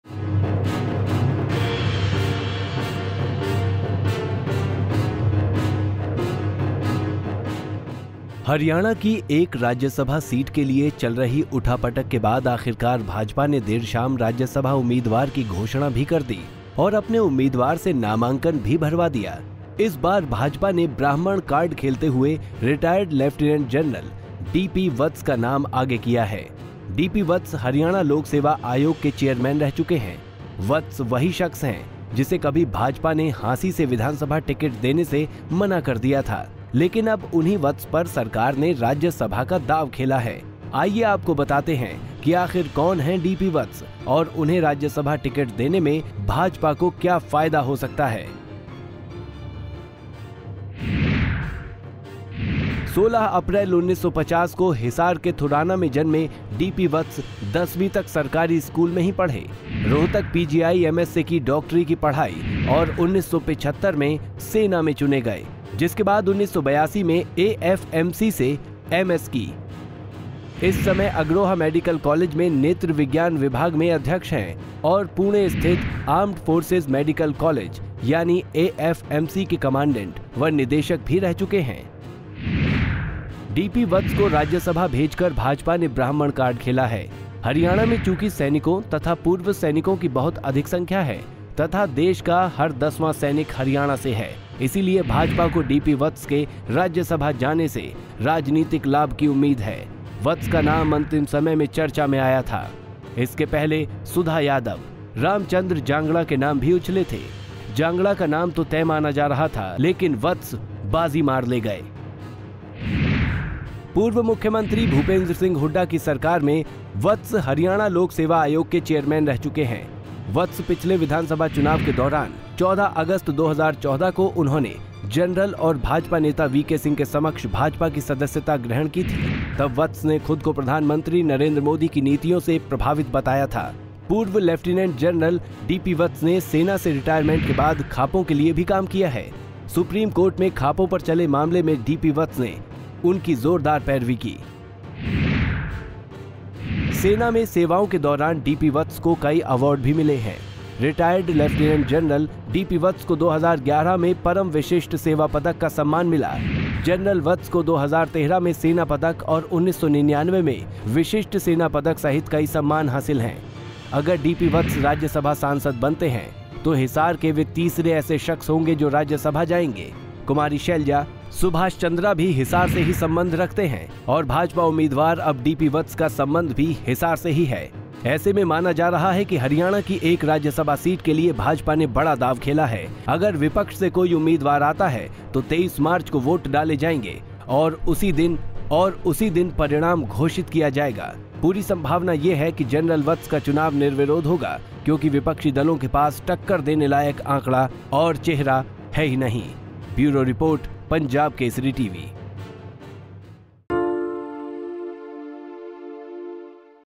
जानिए कौन हैं रिटायर्ड लेफ्टिनेंट जनरल डीपी वत्स, जिस पर BJP ने खेला राज्यसभा दांव || DP Vats For RS
हरियाणा की एक राज्यसभा सीट के लिए चल रही उठा पटक के बाद आखिरकार भाजपा ने देर शाम राज्यसभा उम्मीदवार की घोषणा भी कर दी और अपने उम्मीदवार से नामाकंन भी भरवा दिया। इस बार भाजपा ने ब्राह्मण कार्ड खेलते हुए रिटायर्ड लेफ्टिनेंट जनरल डीपी वत्स का नाम आगे किया है